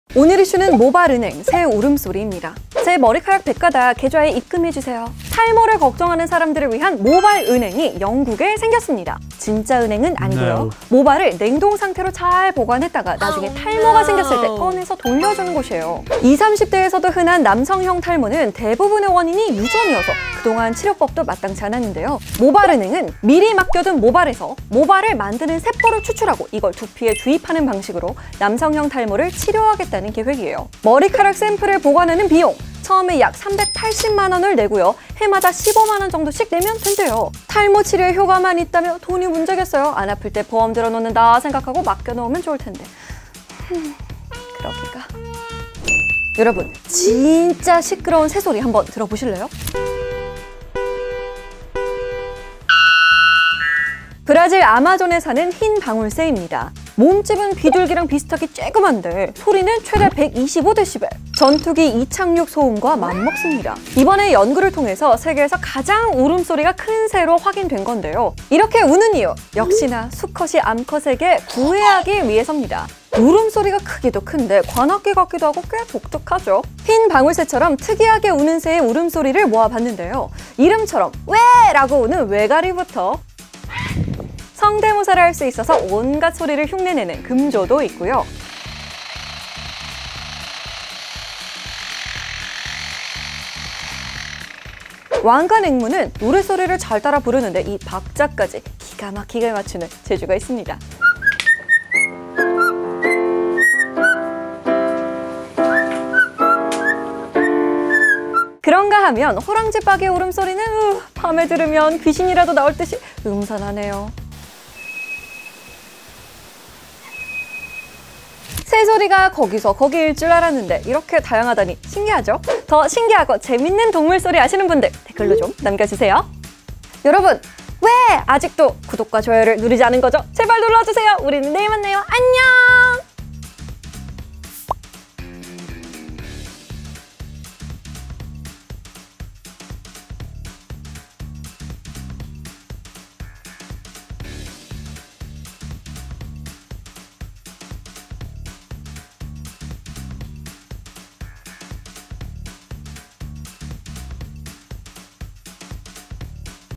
머리카락, 아껴뒀다가 탈모 왔을 때 인출합시다~ / 14F
‘남성형 탈모’를 치료하겠다는 모발 은행이 영국에 생겼습니다. 모발을 냉동상태로 잘 보관했다가 나중에 탈모가 생겼을 때 꺼내서 돌려주는 곳인데요. 여기서 모유두세포를 추출해 탈모를 치료하겠다는 방식이에요. 집안에 탈모 유전자가 있다면 보험 든다 생각하고 알아봐야 하려나요? ㅠ_ㅠ